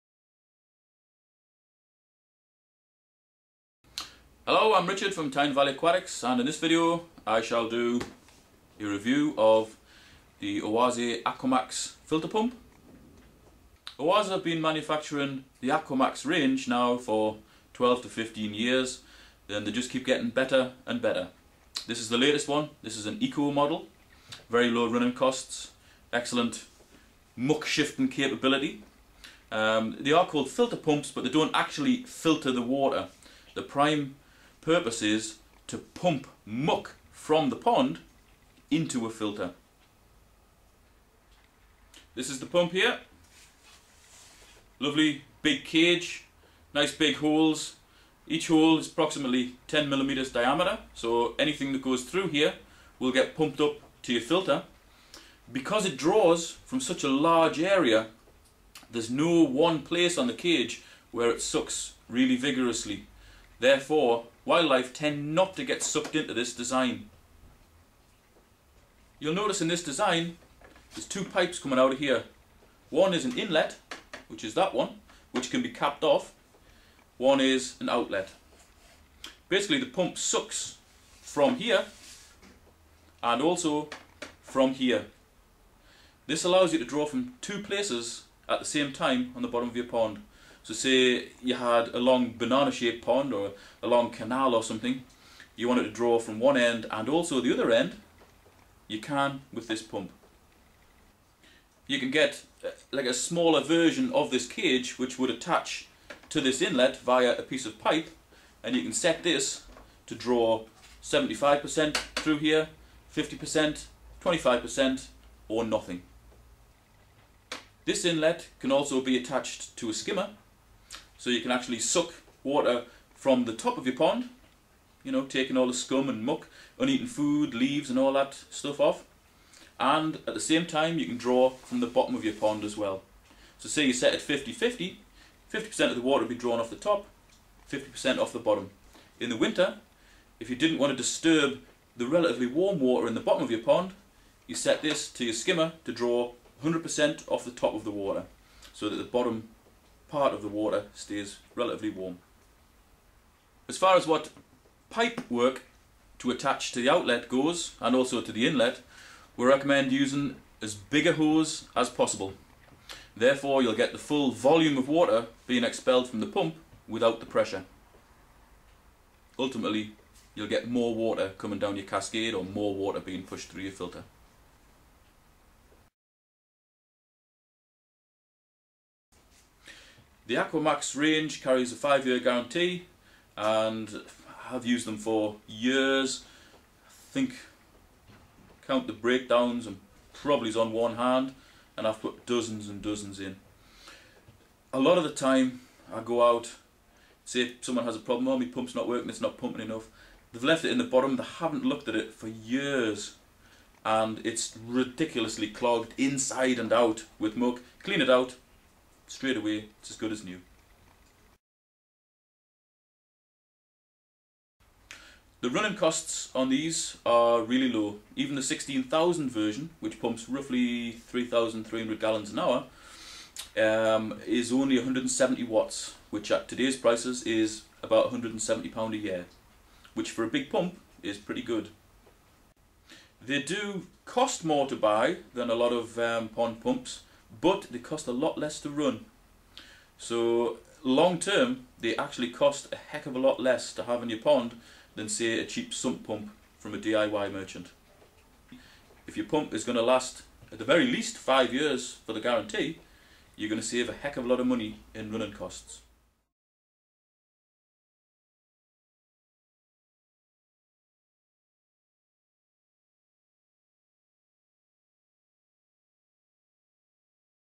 OASE Aquamax Eco Pond Pump Review by Tyne Valley Aquatics
Short video review for the Aquamax Eco pond pump manufactured by OASE. I have used these pumps for many years and they are excellent, shifting large volumes of water to cascades and filters for very low running costs.
The design allows the pump to draw from the cage and also through a skimmer or satellite strainer, thus reducing 'dead spots' on the pond.
5 year guarantee on all models in the aquamax range.
Sold from Tyne Valley Aquatics at competitive prices. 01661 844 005.

For more videos, check the playlist on te right side of the page.